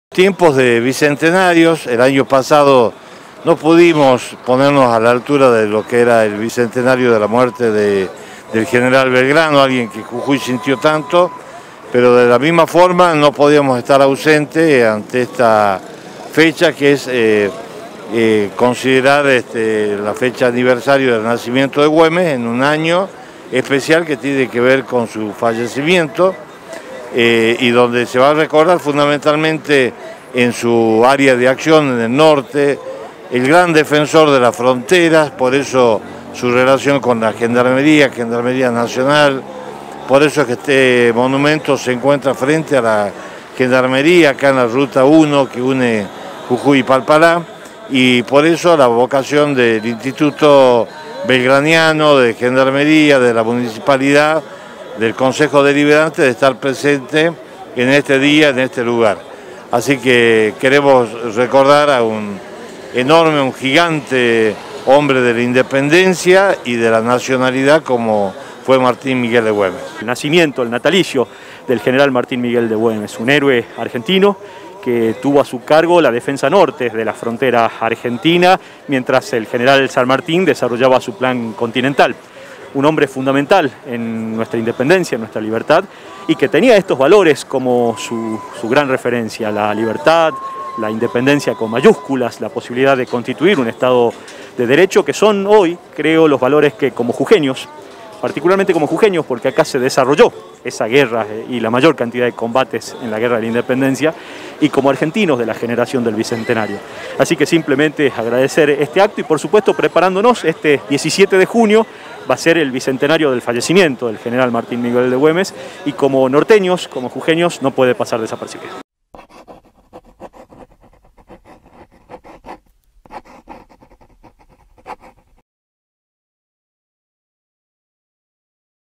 Aniversario del Natalacio del Gral. Martín Miguel de Güemes
En un  acto presidido por el Intendente de San Salvador de Jujuy, Raúl “Chuli" Jorge, se realizó el homenaje y aniversario del natalicio del Gral. Martín Miguel de Güemes en inmediaciones de la Ruta Nro.1,considerándolo  como “un hombre de la Independencia y de la nacionalidad”.